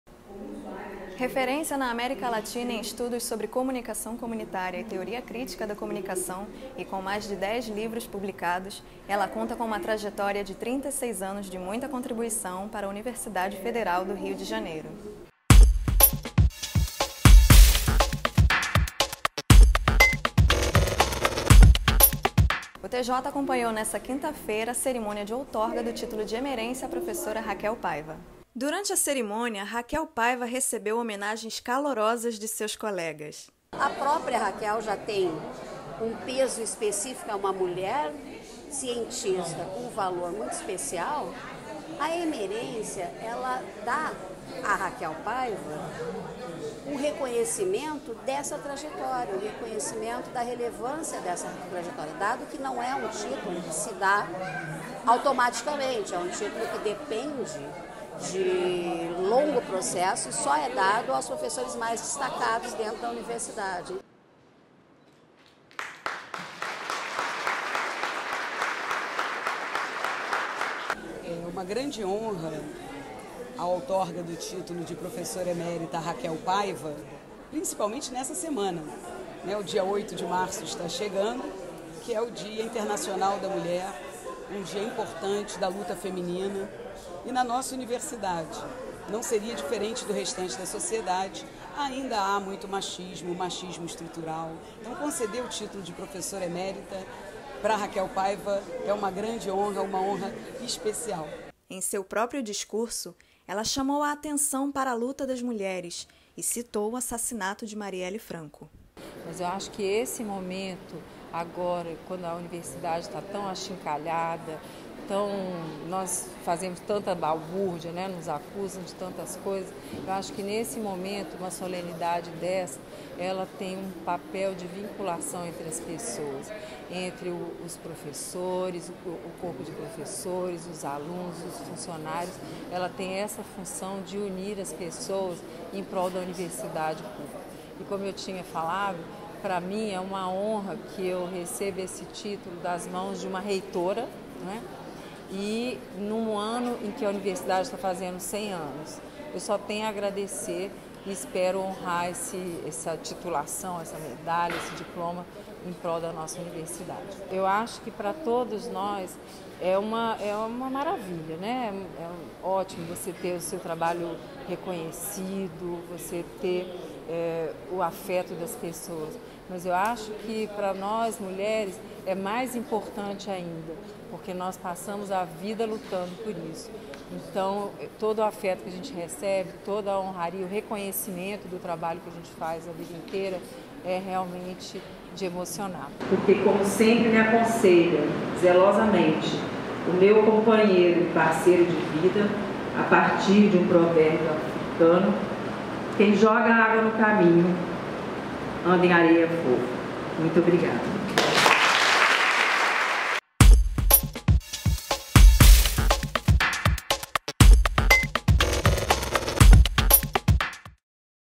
Emerência de Raquel Paiva
Não há forma mais simbólica de passar o dia internacional da mulher que essa: a professora da eco, @paivaraquel referência nos estudos de comunicação, agora também é professora emérita na UFRJ.
Vamos à luta.

A matéria completa você confere no YouTube e no Facebook.